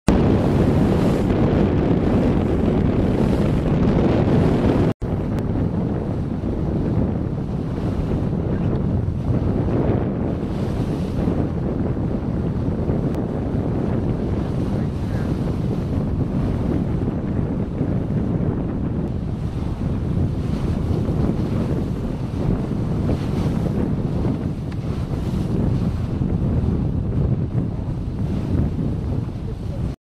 Gray Whale Watch
Watching gray whale during the Archipelago Wildlife Cruise tour in Ucluelet, BC on July 24, 2011.